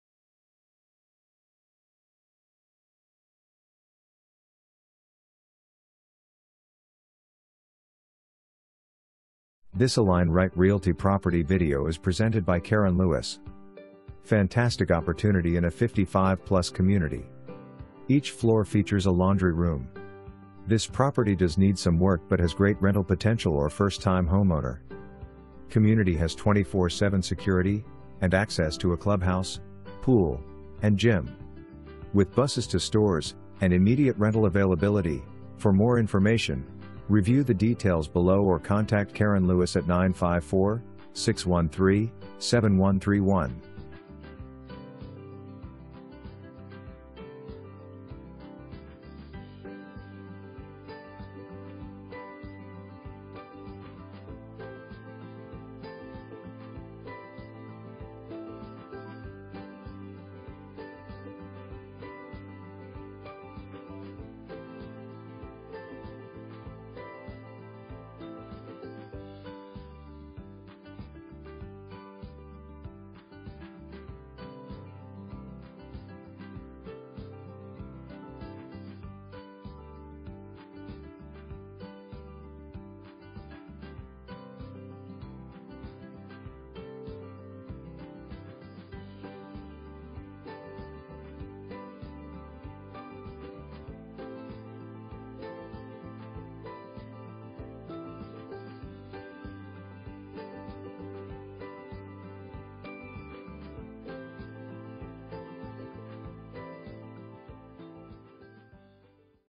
Homes for Sale - 3081 NW 47th Ter #107, Lauderdale Lakes, FL
Fantastic opportunity in a 55+ community! Each floor features a laundry room. This property does need some work but has great rental potential or first time homeowner. Community has 24/7 security, and access to a clubhouse, pool, and gym. With buses to stores, and immediate rental availability,

2 full bath

Karen Lewis
Align Right Realty K1 Group